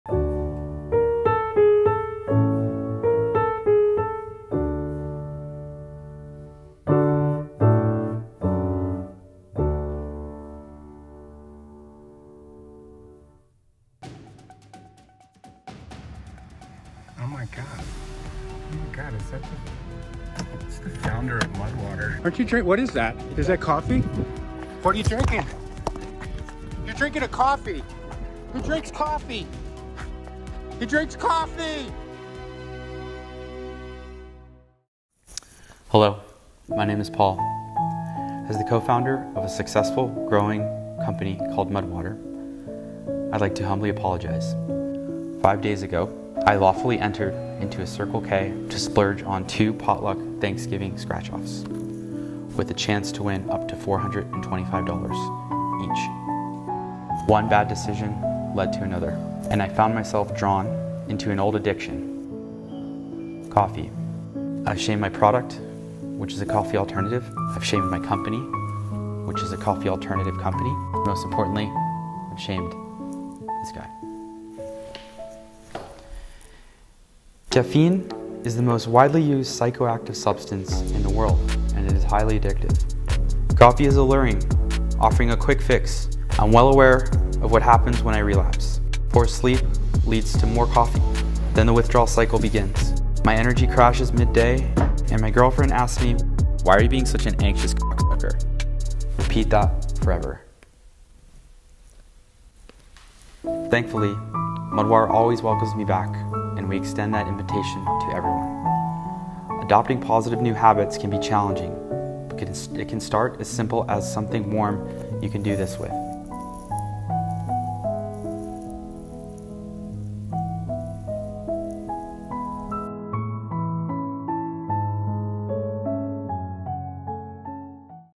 MUD\WTR co-founder caught in SCANDAL!
Last October, MUD\WTR was involved in a filthy scandal: Company co-founder Paul DeJoe was spotted leaving a Circle K in LA, with a gallon-sized cup of coffee in hand. This is not behavior we expect from our founding team. Paul has been reprimanded. Please accept this video as our formal apology. Sorry, APaulogy.